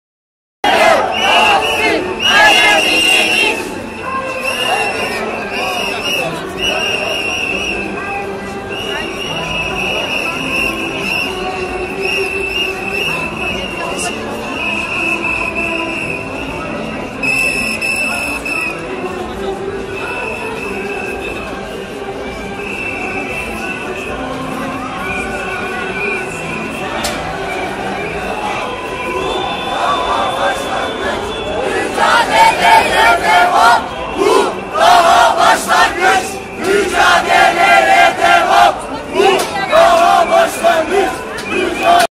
29 ekim 2013 - ankara'da cumhuriyet bayramı yürüyüşü
kızılay'da (sakarya caddesi) çankaya belediyesi binası önünden başlayan meşaleli yürüyüş ulus'taki eski meclis binasına kadar sürdü